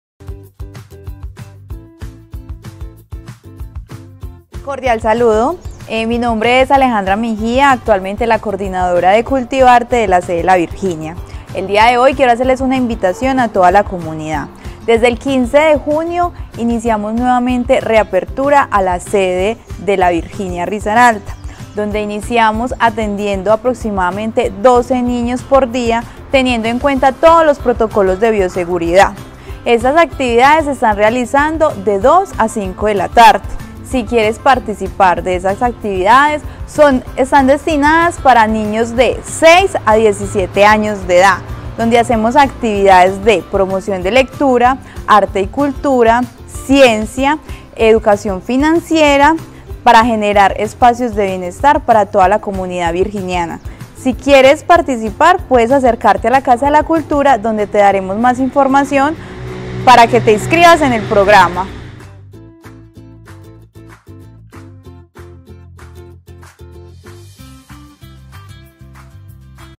INVITACIÓN COMUNIDAD DE LA VIRGINIA.
La Administración Municipal y el Programa Cultivarte de la Casa de la Cultura, invitan a la comunidad a participar de las actividades de bienestar para todos los niños y adolescentes entre los 6 y 7 años de edad, en la promoción de lectura, ciencia, educación financiera, arte y cultura de 2:00 pm a 5:00 pm.
¡Qué esperas para participar en estas actividades!